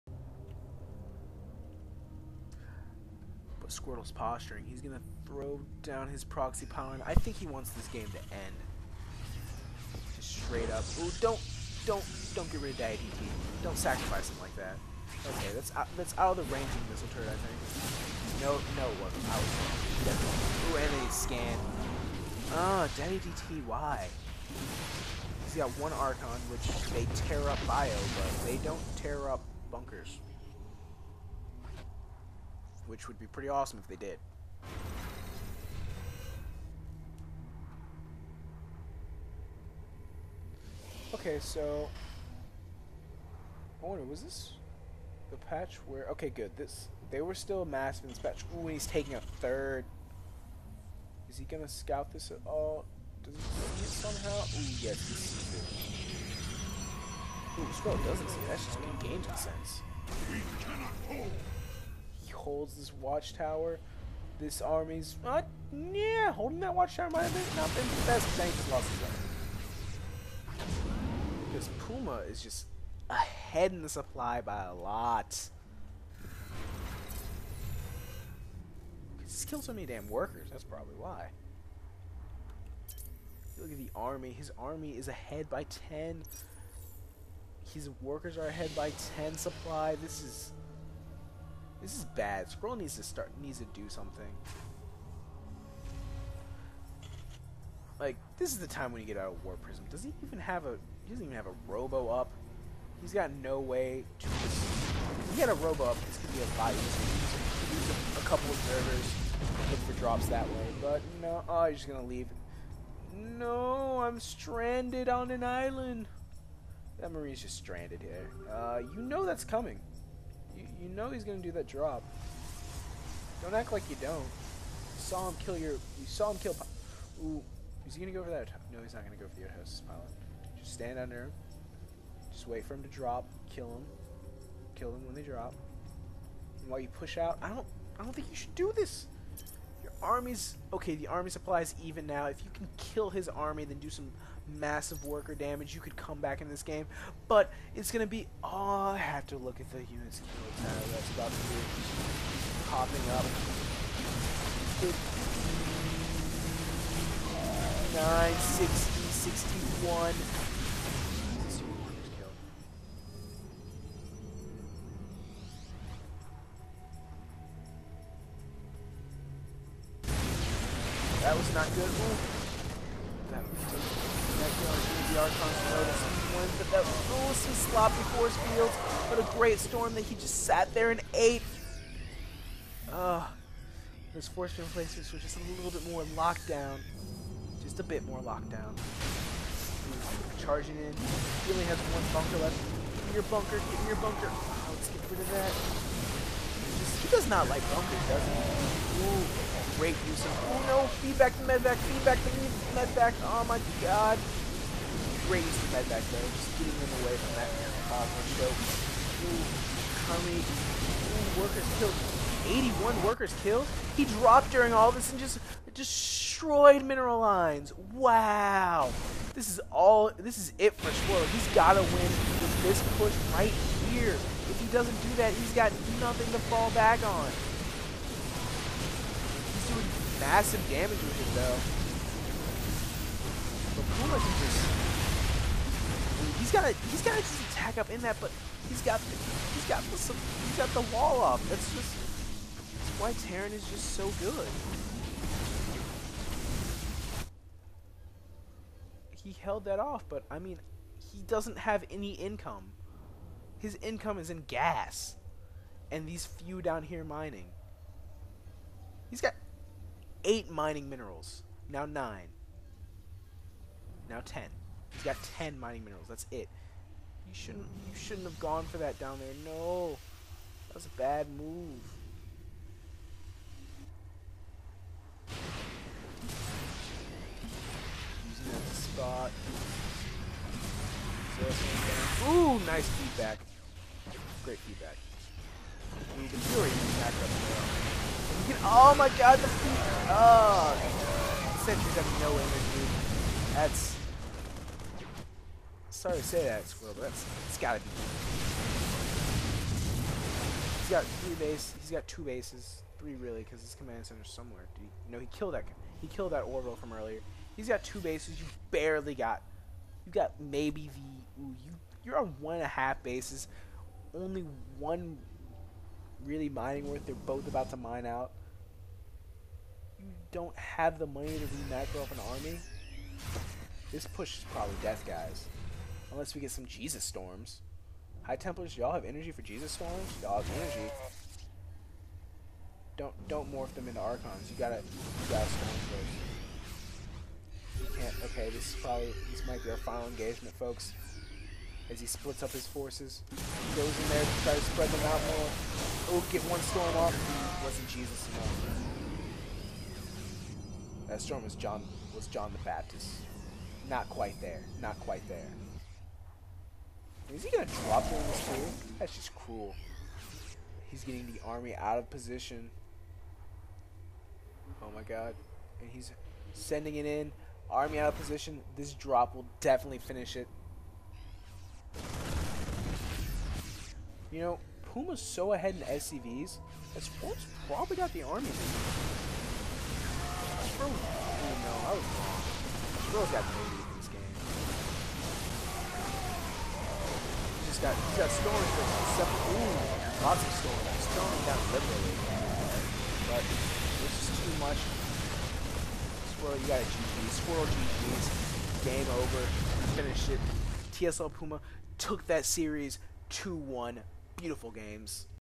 NASL EGPuma[T] V STSquirtle[P] Game 3 3/3
Final part of the exciting conclusion to the battle of these two Korean Pros.